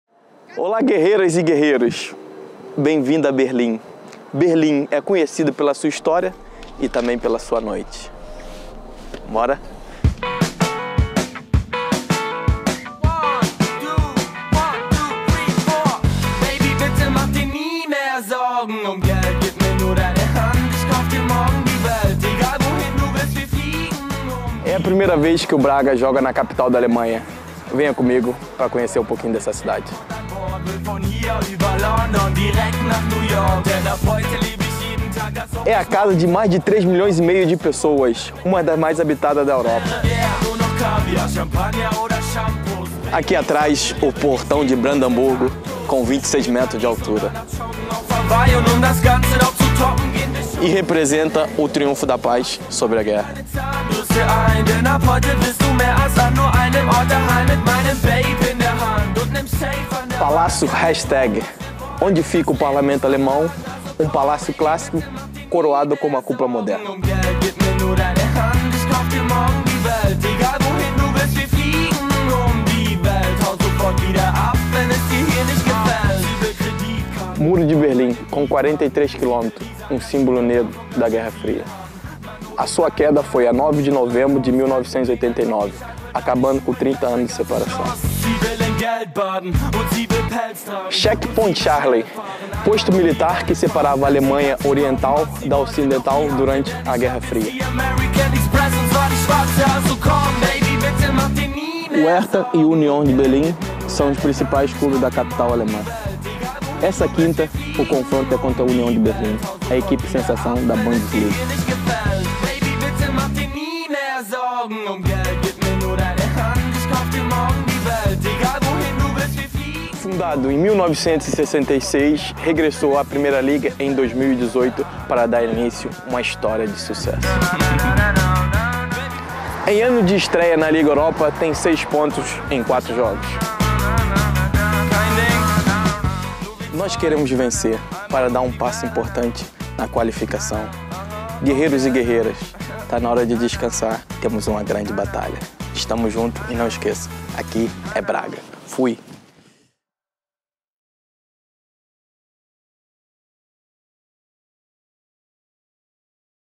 🤴🏾 Alan na Europa | Berlim 🇩🇪
Berlim, cidade de história, igualdade, cultura, techno e agora... uma das cidades que tiveram o privilégio de receber o Rei 🤴🏾 Alan.

Ativem o modo noturno 🌙, é hora de mais um tour.